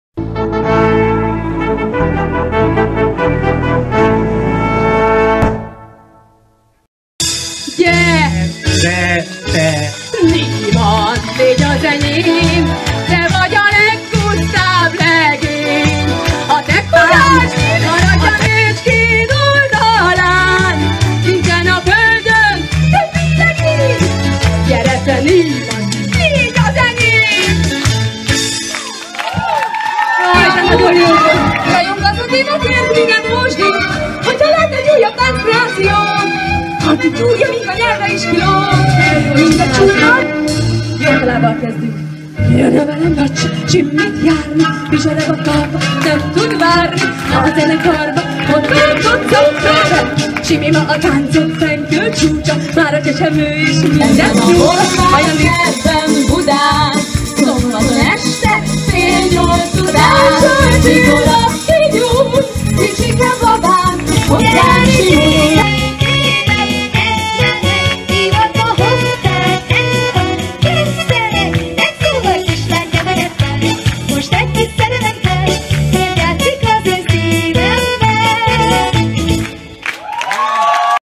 Kis Dóra Demeter Zsuzsi operett egyveleg
Jókedvű egy órás interaktív zenés összeállítás,örökzöldekkel és operettel megfűszerezve.Remek kikapcsolódás időseknek és fiataloknak egyaránt.Ajánlom falunapokra, idősek napjára és mindenkinek aki velünk szeretne eltölteni egy kis nosztalgikus hangulatú órát.